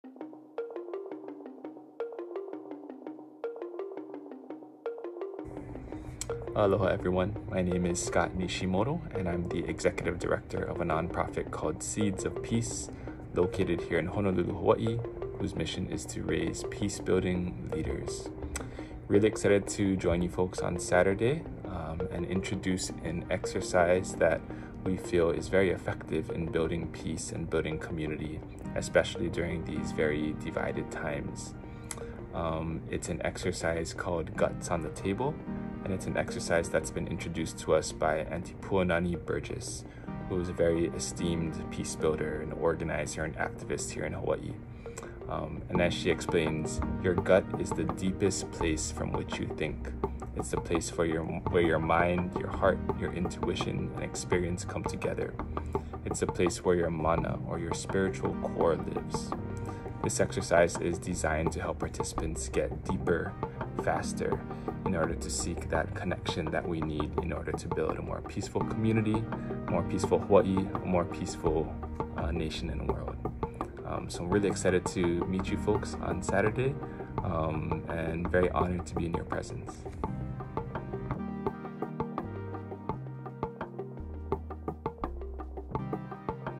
Scott Nishimoto | TEACH IN FOR PEACE | Trailer
The terrible war waged by the Russian government against Ukraine has focused the world’s attention on the horrors of war and the need for peace. Peace does not just happen; it requires sustained effort to create cultures and practices of peace and nonviolence. One important component of this is education. The purpose of this teach-in is to share practices for and approaches to education for peace and ahimsa.

WHAT IS A TEACH-IN?
The concept of a teach-in was developed in the United States in the 1960s as a way to engage people in learning about and discussing complex topics such as war and oppression. They typically include lectures, discussions, and engaging educational activities that further participants’ understanding of the issue at hand. Until recently most teach-ins have taken place in person but the availability of technology coupled with the experience of online engagement since the beginning of the pandemic has allowed for the possibility of a virtual teach-in.

On Saturday, April 23, 2022, Speaking Our Peace and Jai Jagat International invite you to join an international Teach-In for Peace, in which you will learn from various practices of education for peace and ahimsa from formal and informal approaches. Presenters will be coming from several countries including Argentina, Palestine, South Africa, India, Canada, and United States.